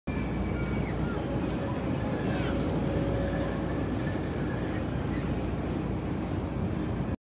This what I did tonight I went to the world 🌎 of lights tonight
It was a lot of fun going to the world of lights